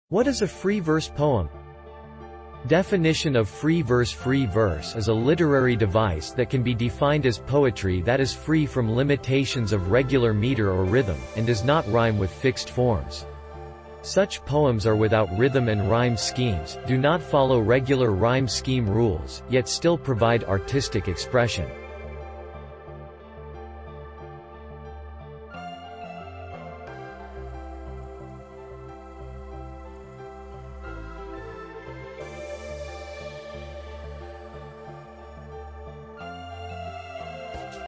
What is a free verse poem?
Definition of Free Verse Free verse is a literary device that can be defined as poetry that is free from limitations of regular meter or rhythm, and does not rhyme with fixed forms. Such poems are without rhythm and rhyme schemes, do not follow regular rhyme scheme rules, yet still provide artistic expression.